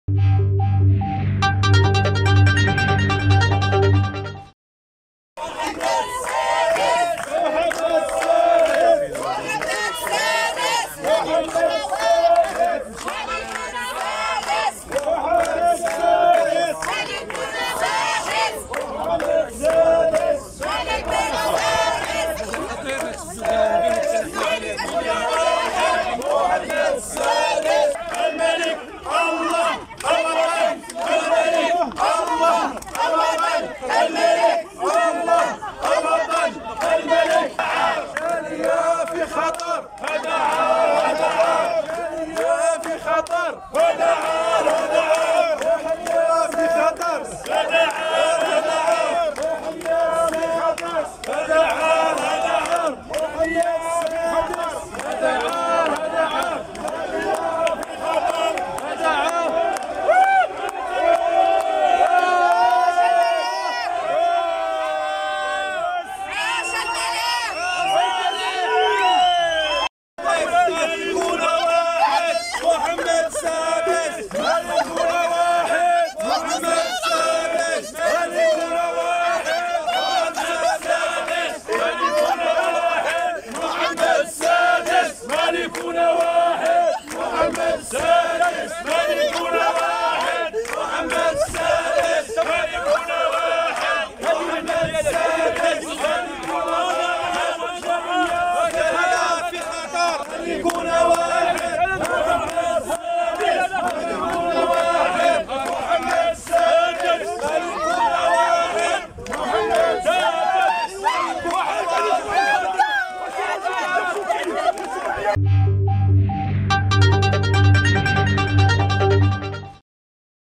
مؤلم..شوفو بكاء وانهيار المغاربة العالقين بالخزيرات بسبب كورونا
رصدت شوف تيفي الحالة التي كان عليها مغاربة عالقون بالخزيرات بسبب تعليق الرحلات.